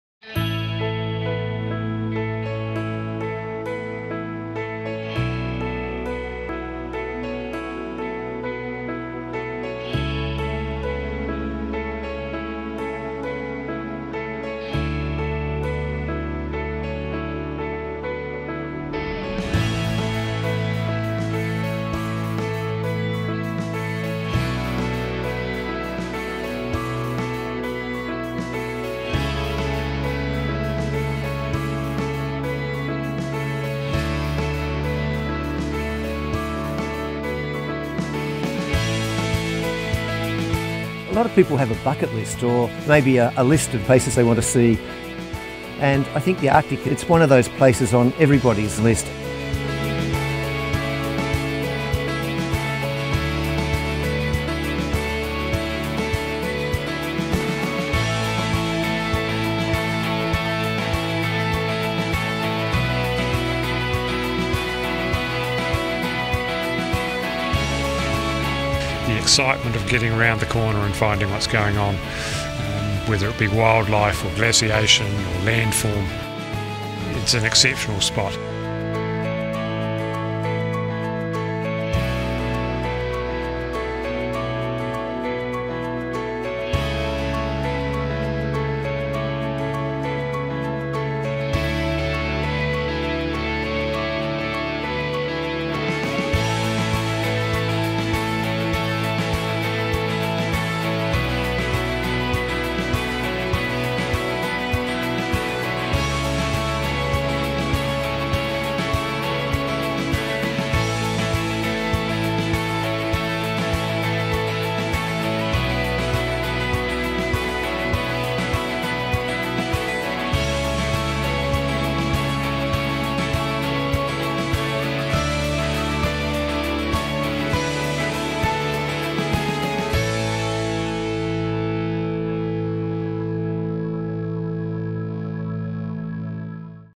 Discover the European Arctic
Jump on board 54-passenger Polar Pioneer and explore a world filled with pristine fjords, thundering glaciers, majestic icebergs and spectacular wildlife!